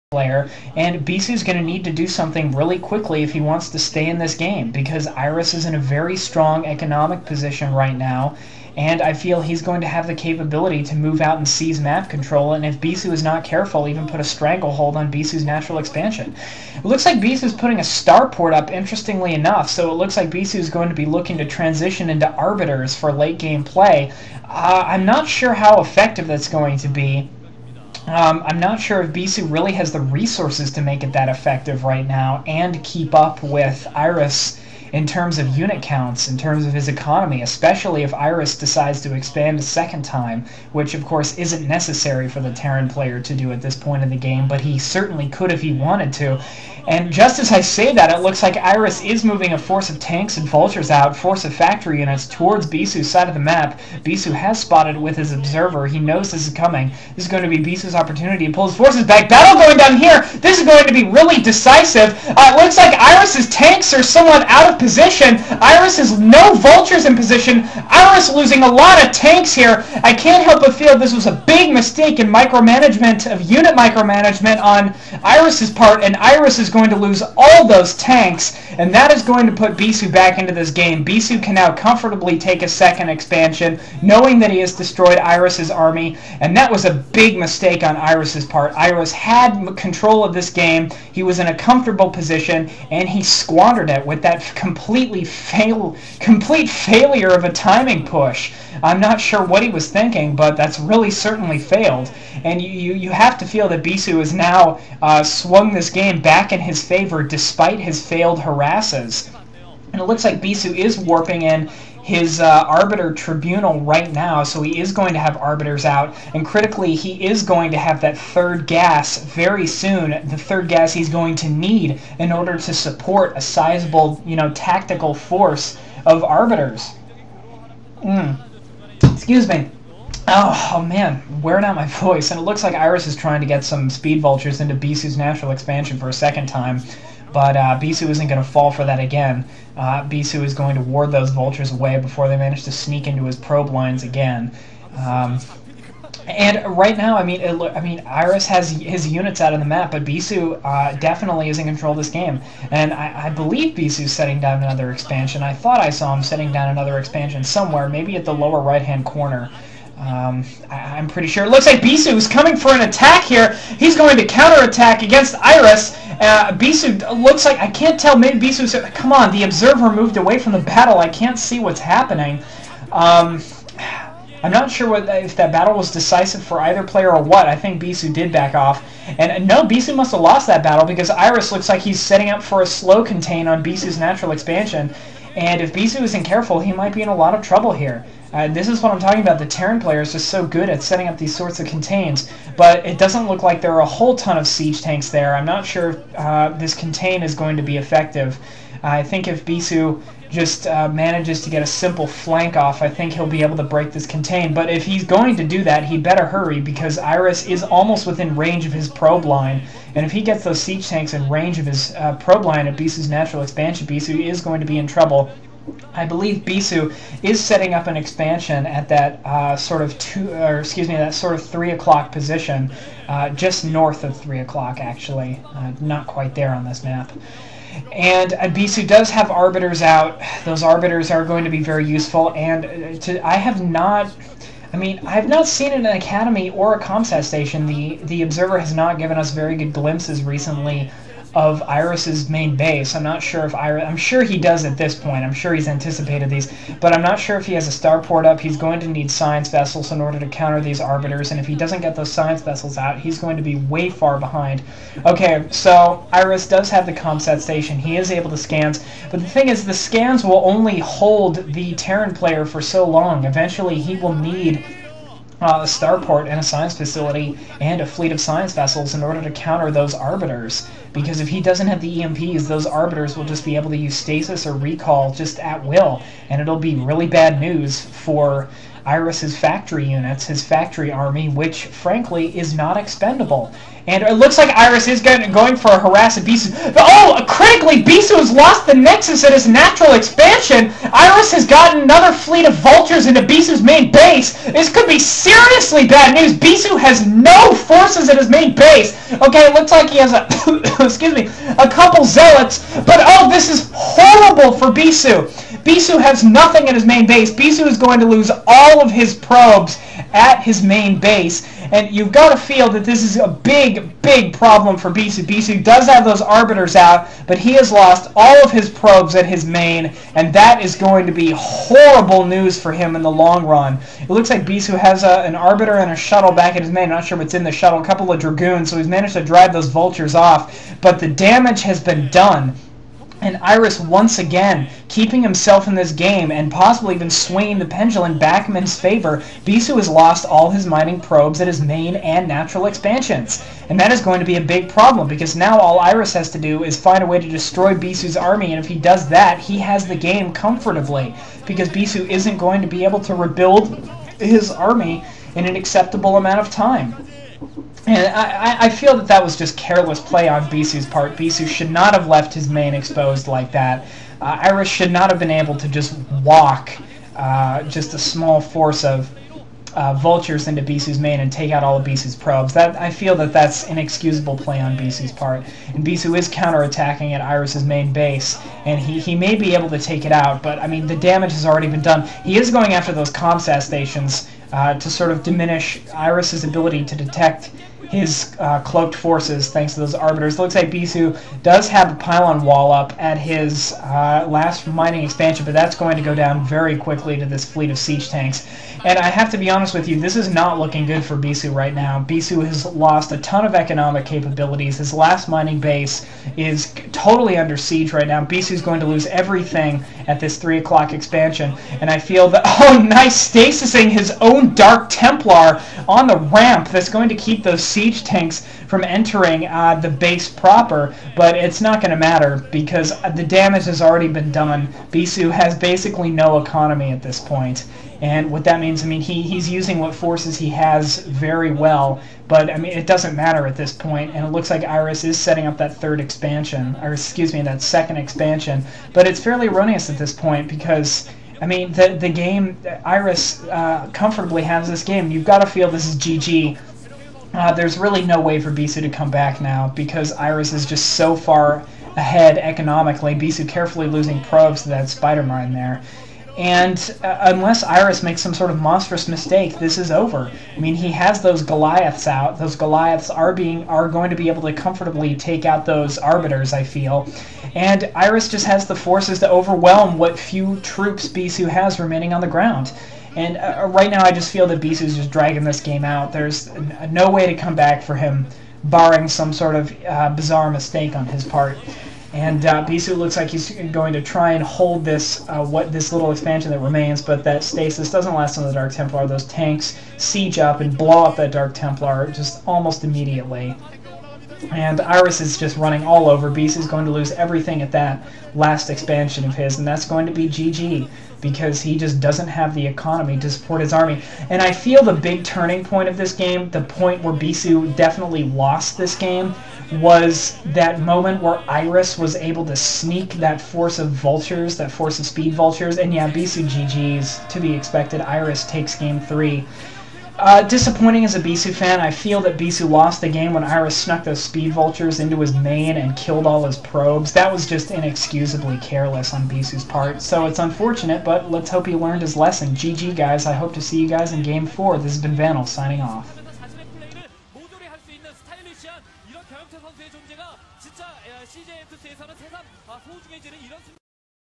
ShinHan Bank Winners League 2008-09: CJ Entus versus SKT T1 Game 3 PART 2
ShinHan Bank Winners League 2008-09: CJ Entus versus SKT T1 Game 3
Iris: Red Terran
Bisu: Tan Protoss
Map: Neo Harmony